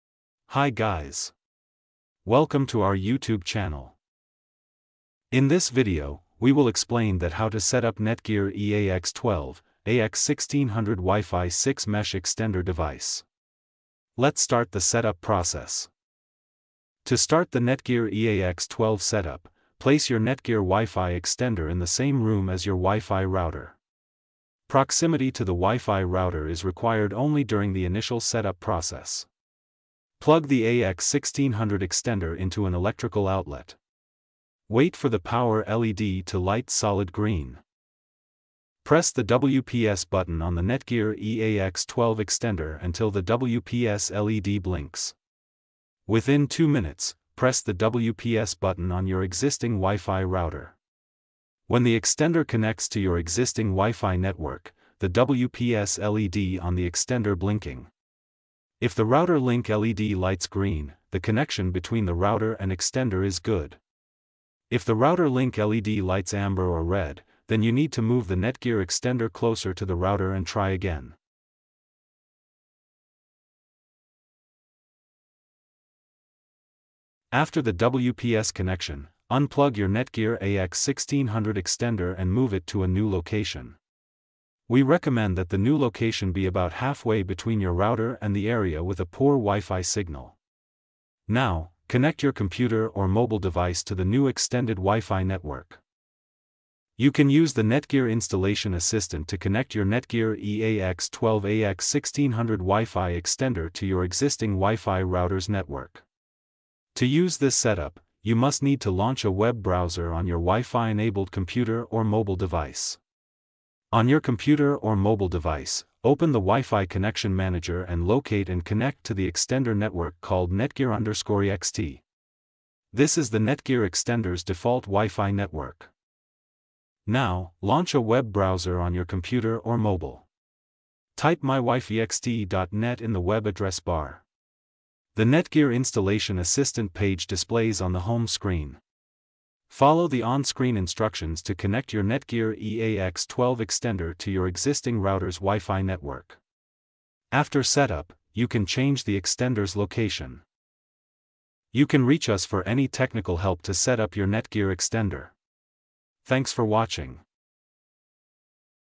How to Setup NETGEAR EAX12 AX1600 WiFi 6 Mesh Extender?
The NETGEAR EAX12 AX1600 WiFi 6 provides 1.6Gbps total combine speed on its 2.4GHz and 5GHz bands. This will helps you to streaming 4K/8K video and online gaming. It extends your existing WiFi network, and actual speeds will vary based on factors like your router, network traffic, and building walls. In this video, we will provide the Setup forNETGEAR EAX12 AX1600 WiFi 6 Mesh Extender device.

Chapters-
0:20 "NETGEAR EAX12 placement for initial setup"
0:45 "NETGEAR EAX12 AX1600 WPS Setup"
1:14 "Contact for technical help"
1:40 "Web Browser Setup"
2:22 "Create a password for dual band Network"

This video will helps you to setup your NETGEAR AX1600 wifi6 extender setup.